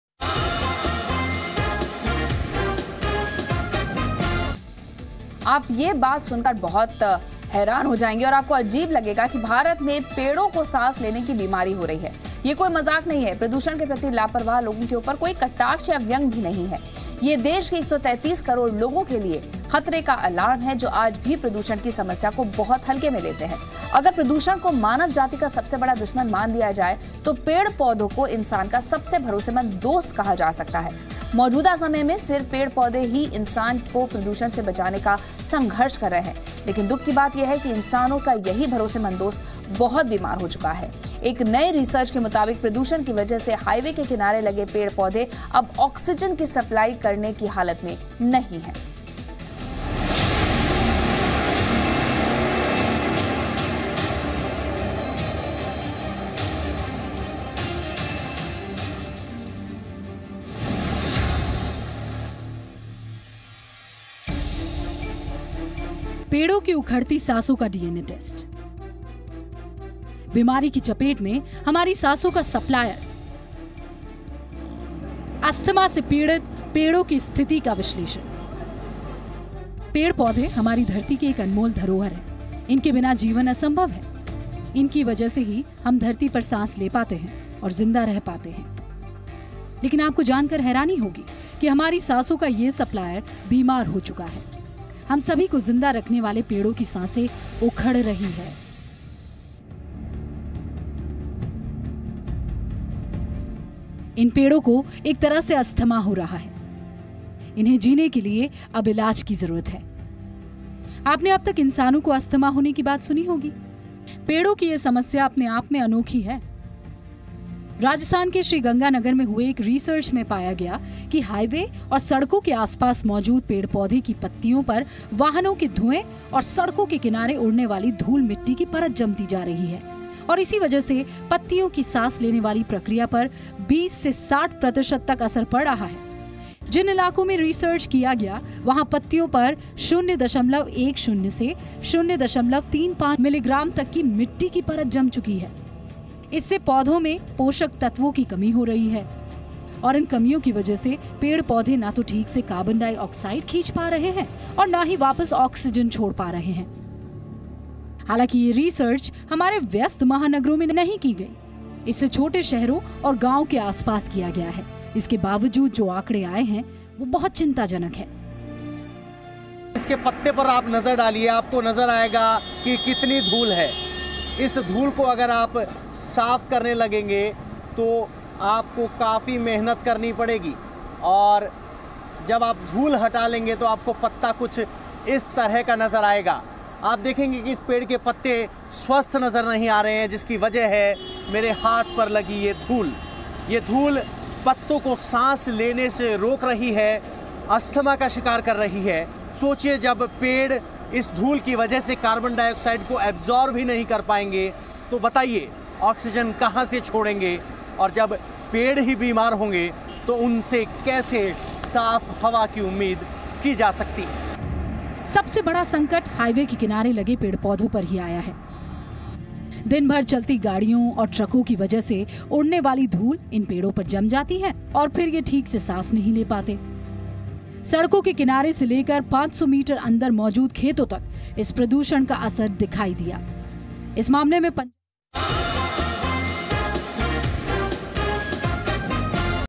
Trees and shrubs on Highways incapable to provide oxygen due to excessive pollution: Research
Roadside trees and shrubs incapable to provide oxygen due to excessive pollution: Research. Watch the video to get more information on news.